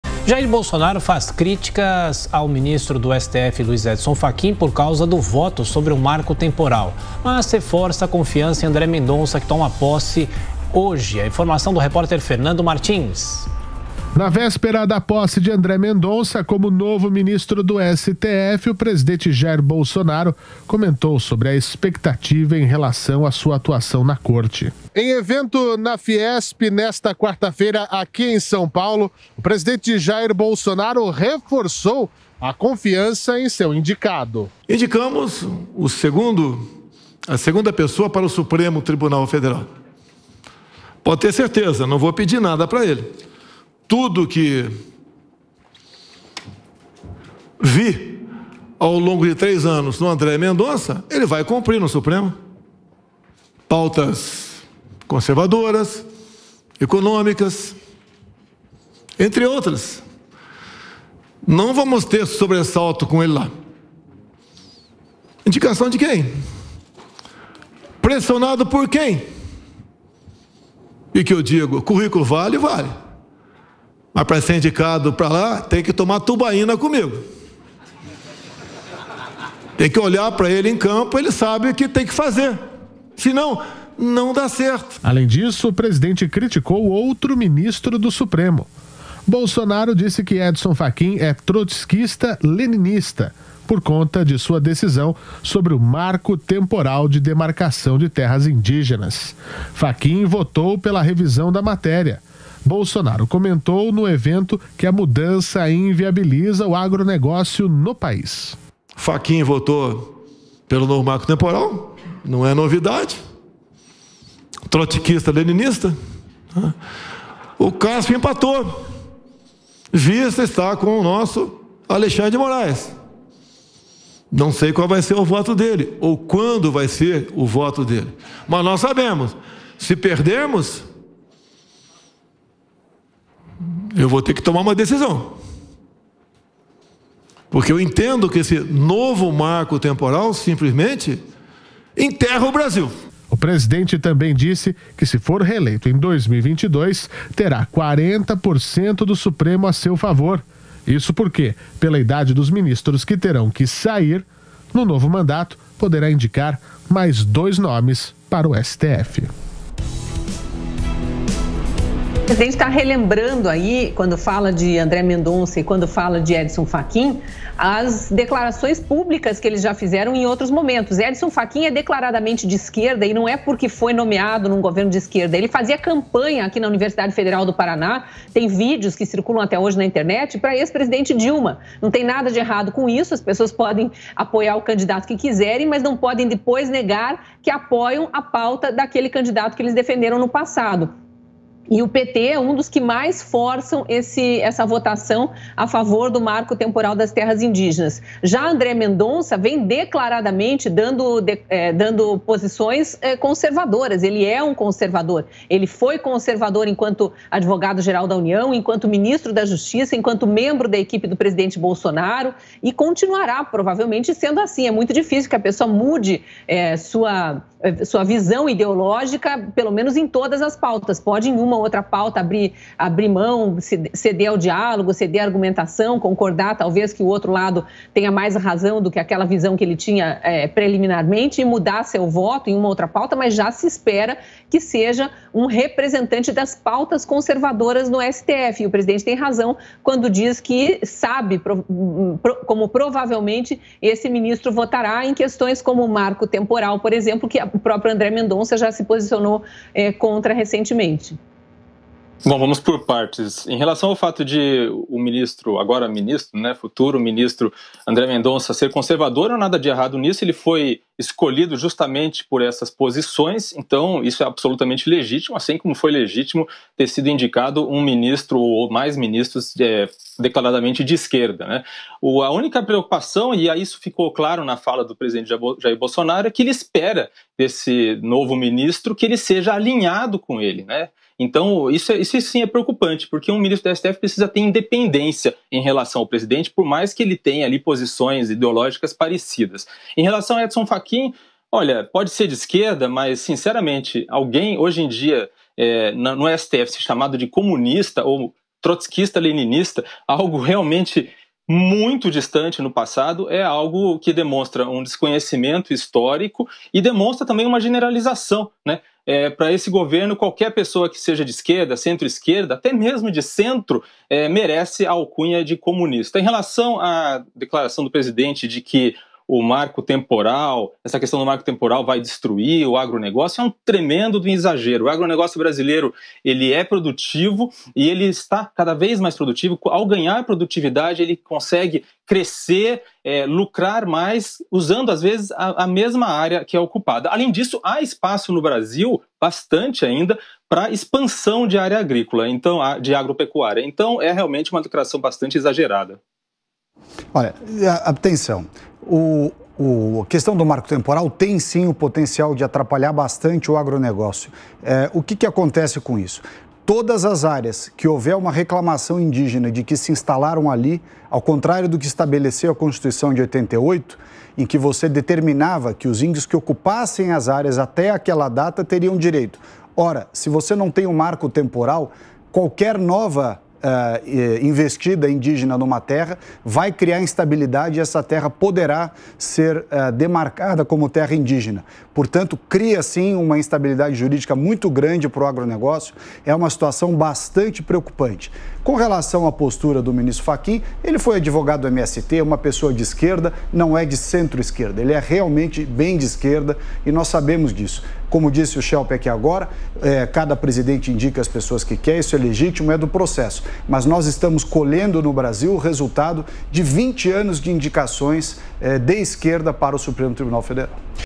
Bolsonaro faz críticas a Fachin após voto em projeto do marco temporal
Na véspera da posse de André Mendonça como novo ministro do STF, o presidente Jair Bolsonaro comentou sobre a expectativa em relação a sua atuação na Corte. Bolsonaro reforçou a confiança em Mendonça e fez críticas contra Fachin em voto de projeto do marco temporal.